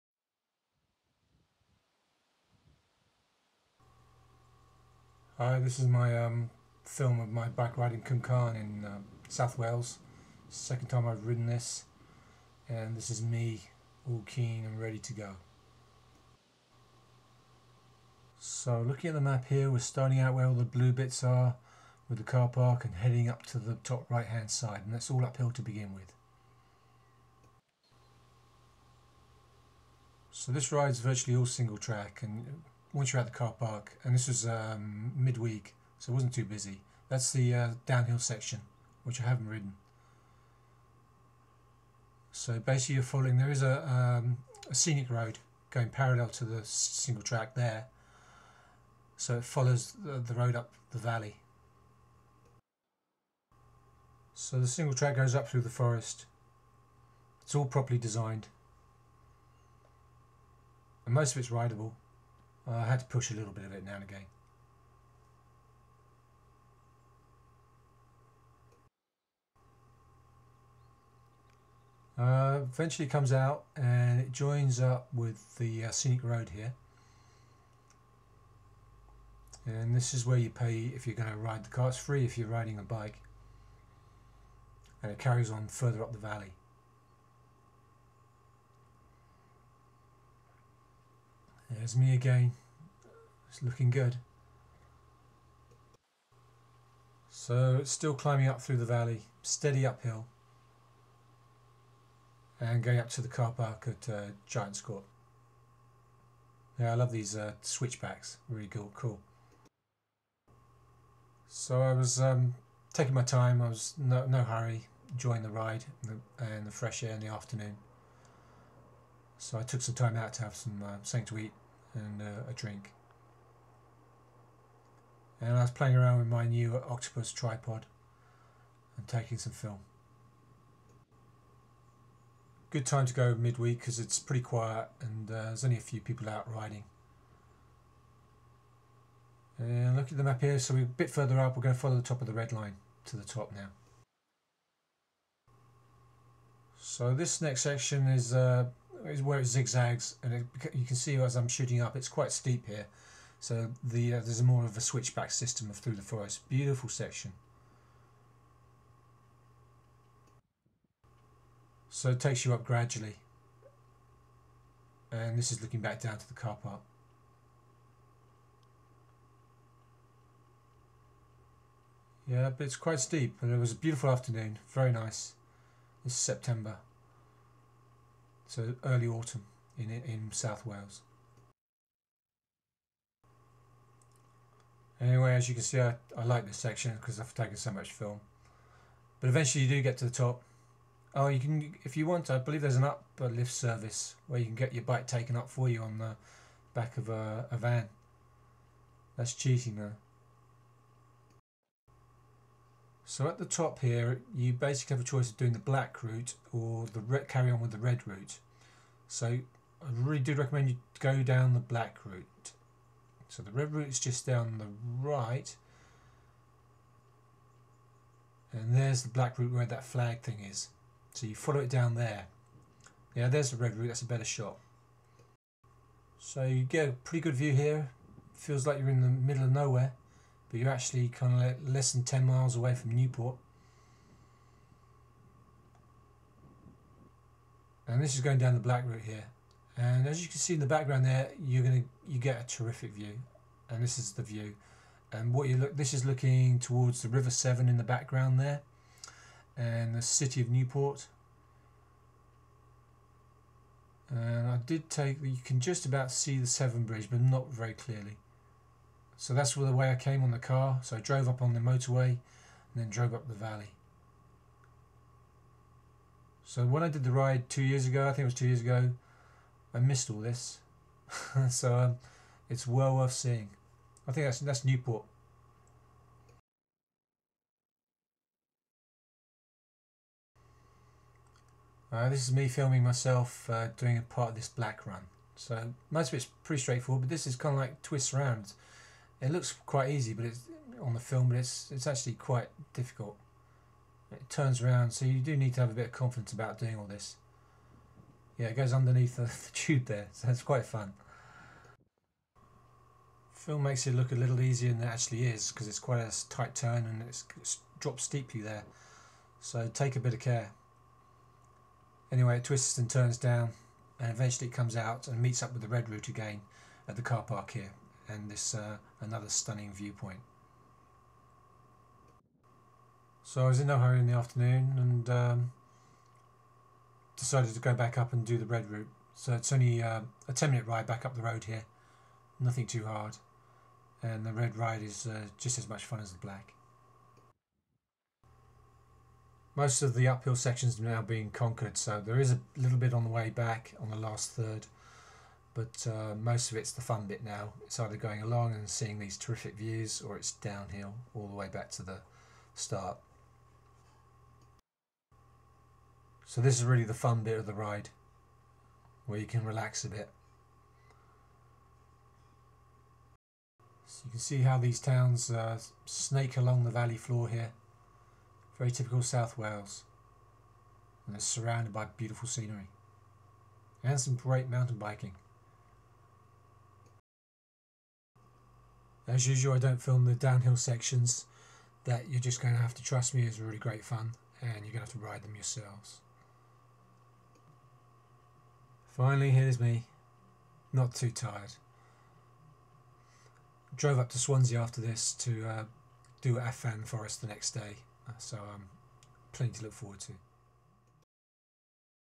Cwmcarn 2009 (Mountain Biking in HD)
Me biking this classic South Wales course. great scenery + me!